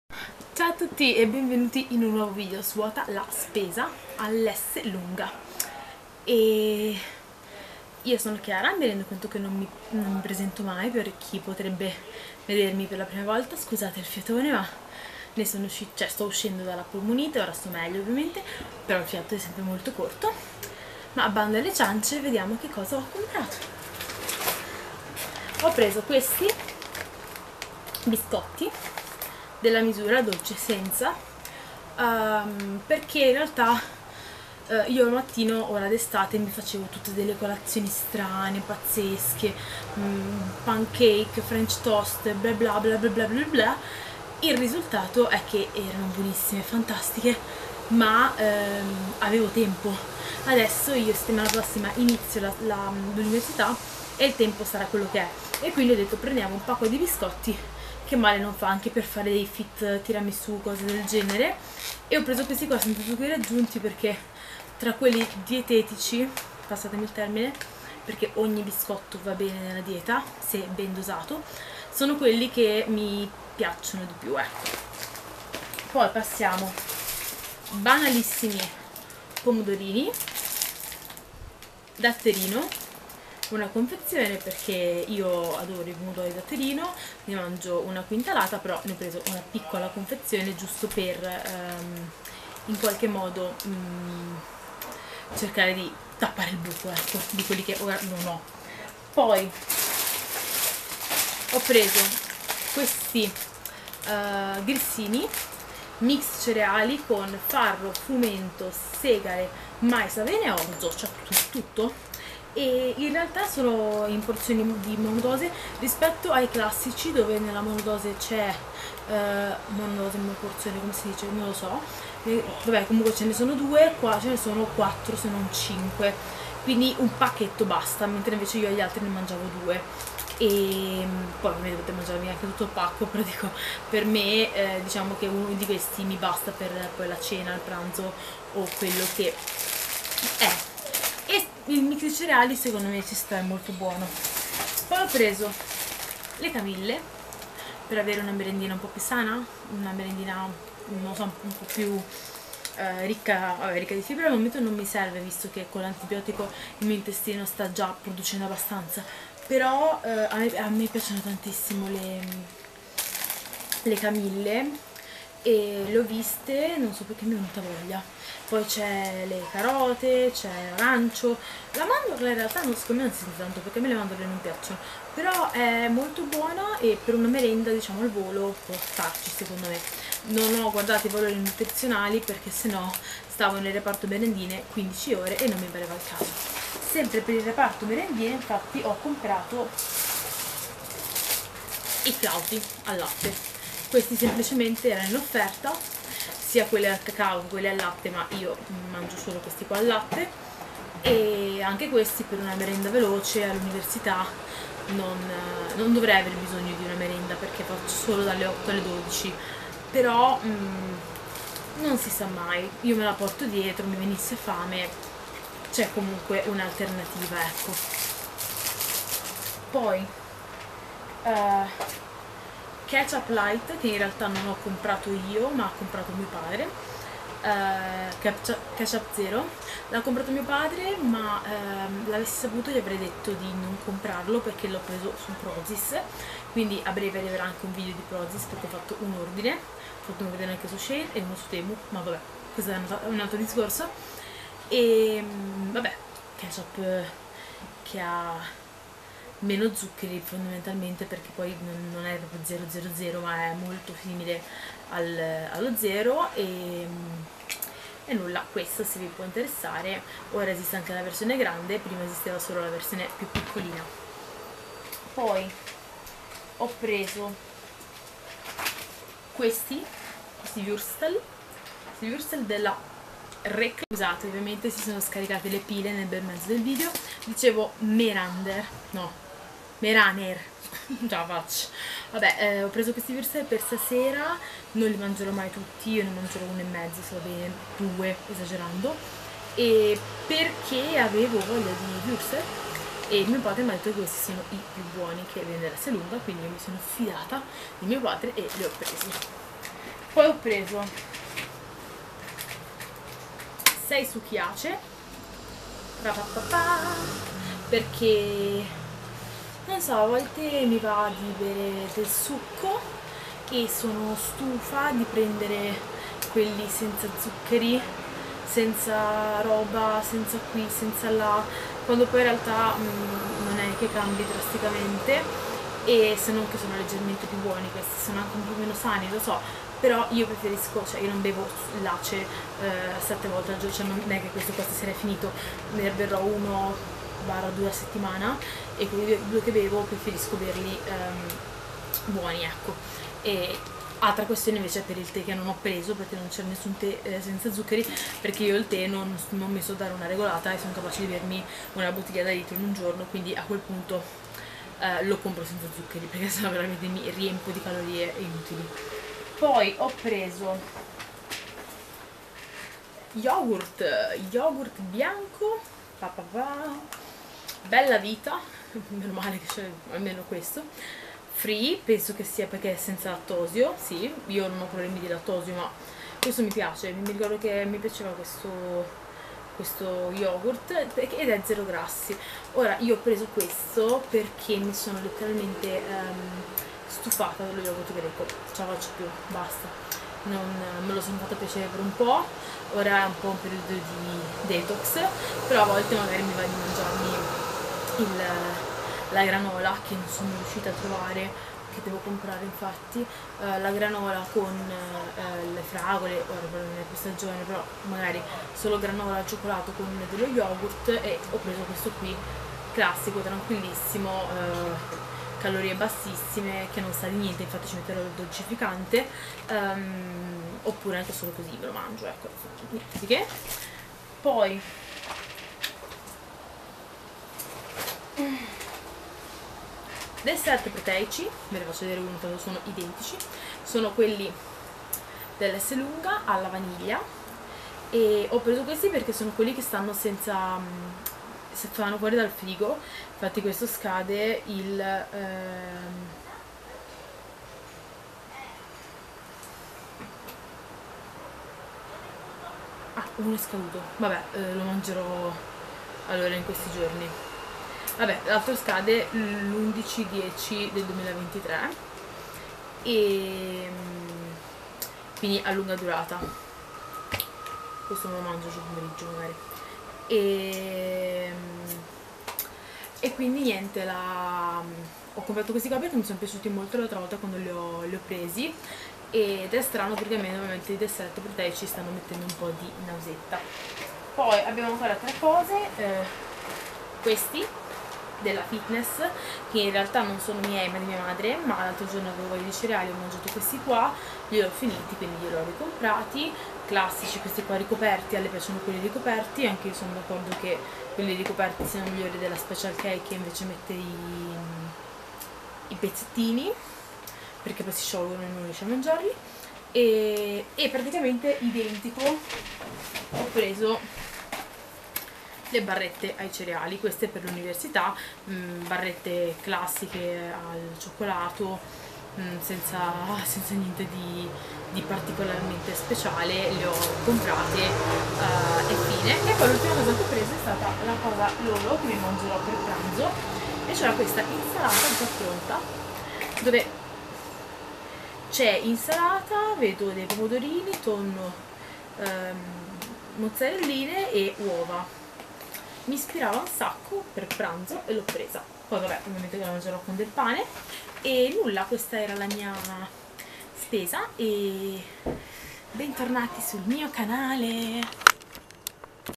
svuota la spesa esselunga
Ciao a tutti e benvenuti in un nuovo svuota la spesa! Spero vi piaccia.

tik tok e ig: momalucilla
vinted: momalucillaa